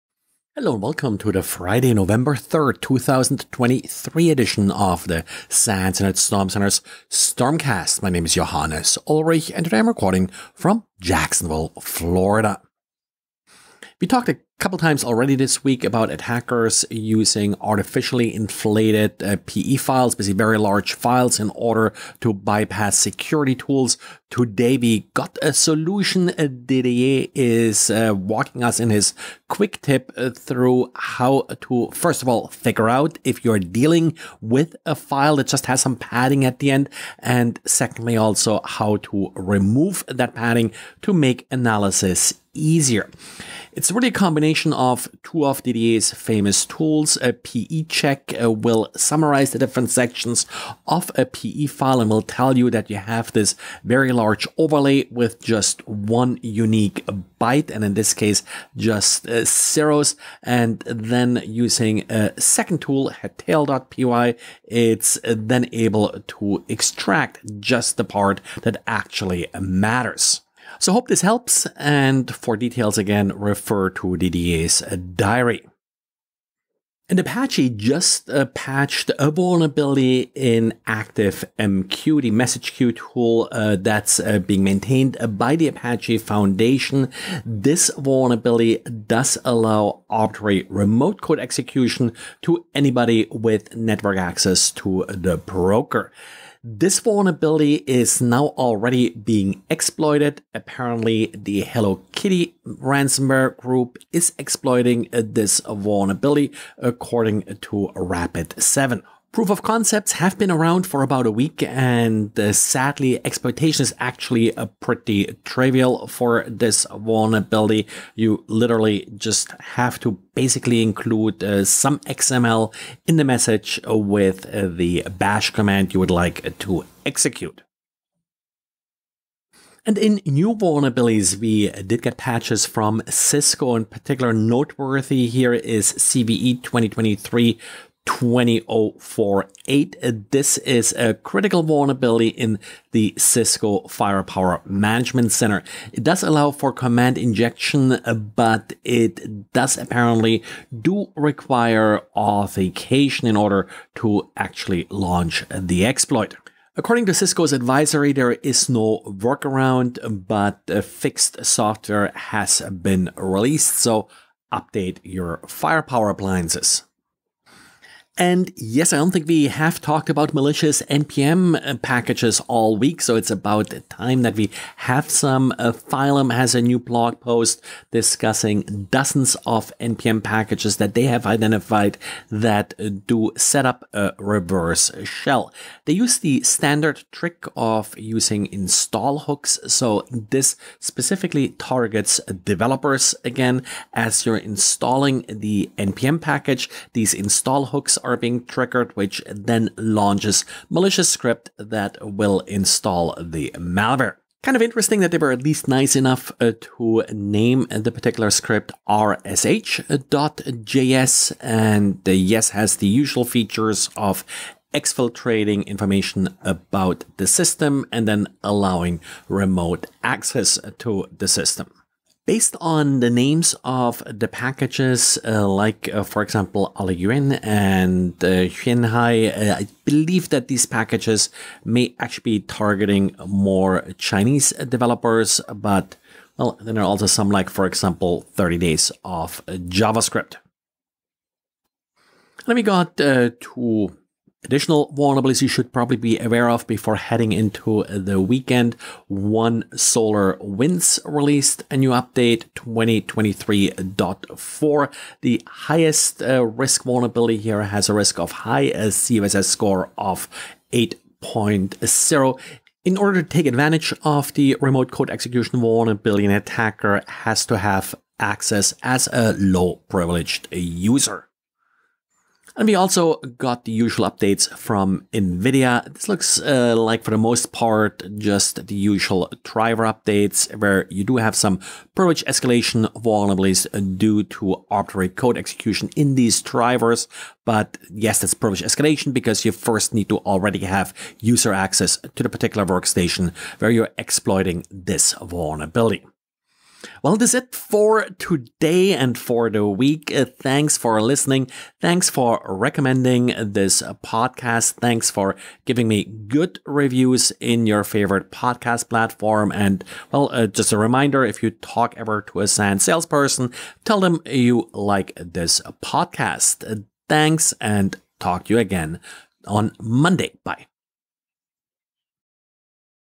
Network Security News Summary for Friday November 03th, 2023
Inflated PE Files; ActiveMQ Exploit; Firepower Vuln; Malicious NPM;

Dozens of npm Packages Caught Attempting to Deploy Reverse Shell